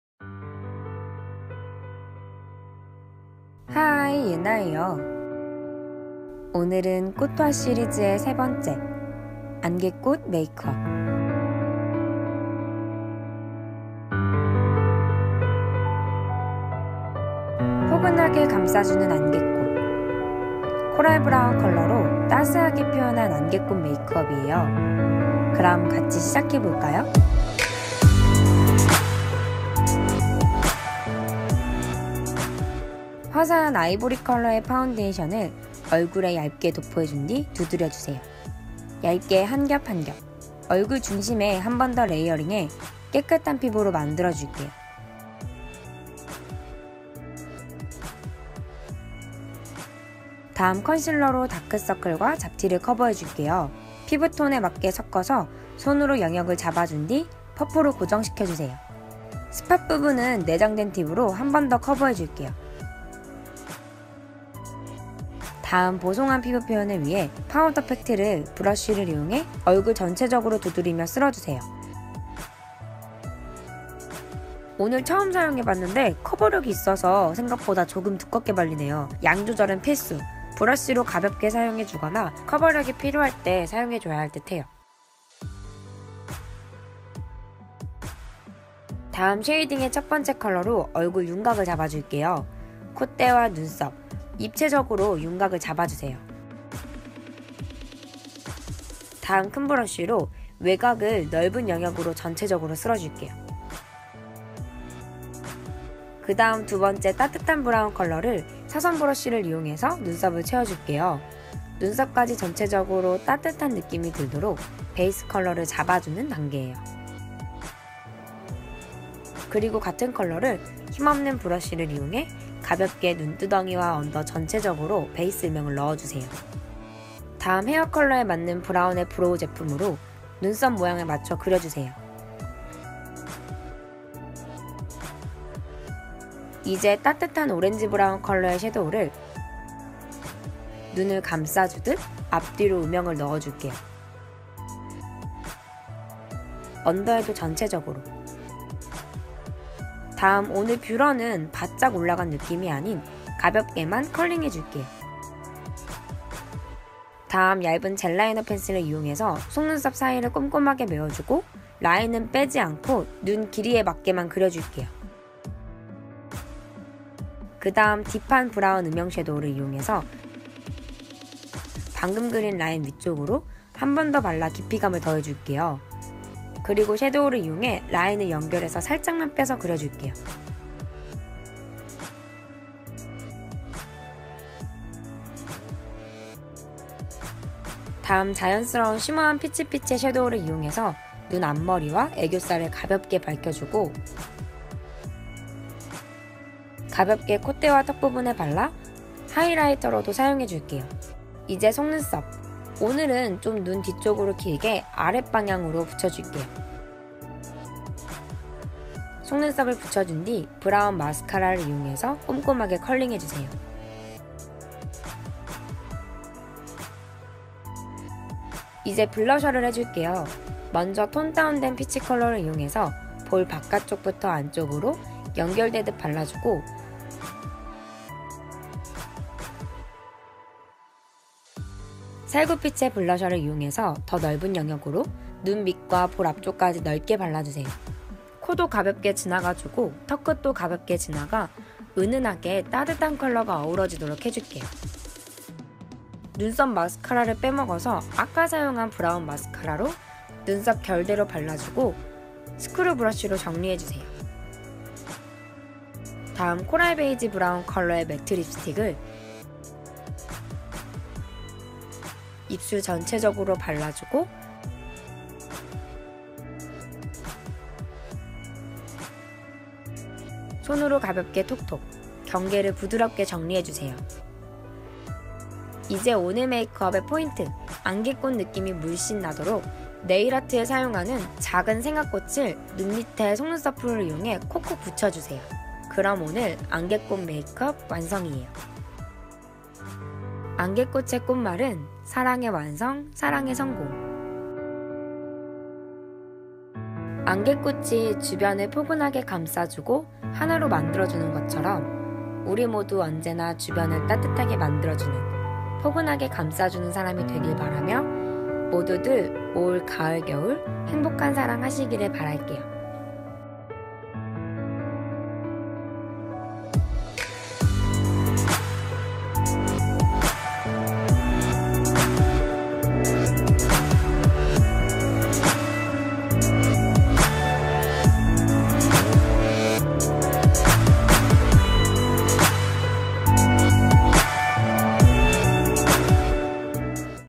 꽃 花 시리즈 part#3 안개꽃 메이크업 l gypsophila make up l hiyena하이예나
오늘은 꽃 花 시리즈 3탄!
안개꽃 메이크업이에요

너무 오랜만에 꽃 花 영상이네요
제가 너무 늦었죠ㅠㅠ!

조금 고민이 많이 되서..그래도! 이제 다시!
시리즈를 이어가보도록 하겠습니당!

기다려주셔서 고마워요❤️

안개꽃의 꽃말은 사랑의 완성, 사랑의 성공

안개꽃이 주변 꽃을 포근하게 감싸주고 하나로 만들어주는 것 처럼

우리 모두 언제나 주변을 따뜻하게 만들어주는

포근하게 감싸주는 사람이 되길 바라며

모두들 올 가을 겨울 행복한 사랑 하시기를 바랄게요!

의상은 2년전..너무오래전에 구매한거라 정보가 없어요ㅠㅠ

네일 생화는 홍대 트로피칼네일 내사랑 써연찡 제공❤️언니고마웡❤️

ESTEE LAUDER
더블웨어 누드 워터 프레시 메이크업
본 / 30ml
68,000원

CANMAKE
컬러 믹싱 팔레트 컨실러
2호 / 3.9g
13,000원

e-glips
커버파우더 팩트
21호 / 10g
17,000원

BANILA CO
더시크릿 페이스 앤 헤어 멀티 쉐이딩
12g
28,000원

BOBBI BROWN
아이섀도우
토스트 / 2.5g
35,000원

The Rose of Versailles
프린세스 앙뚜아네뜨 젤펜슬라이너
브라운 / 0.14g
20,000원

gesgep
아이파노라마
EARTH, BRICK / 2.4g
20,000원

NARS
듀오 아이섀도우
실크로드 / 4g
49,000원

KISS ME
히로인 메이크 롱앤컬 마스카라
S 브라운 / 6g
18,000원

CLINIQUE
치크팝
핑크허니팝 / 3.5g
30,000원

CANMAKE
파우더 치크
슈가오렌지 / 3.6g
11,000원
WAKEMAKE
립 코스터 M
베이지 페리 / 4.5g
15,000원

espoir_블랜딩퍼프

THE TOOL LAB_152
CANMAKE_컨실러 팔레트 내장 브러쉬
piccasso_206a
THE TOOL LAB_201
MUSTAEV_E93
MAKE UP FOR EVER_160
THE TOOL LAB_203
MUSTAEV_E13
MUSTAEV_E27
the Balm_파우더 브러쉬
LUNA_사선브러쉬(카이콜라보)

piccasso_래쉬걸 S01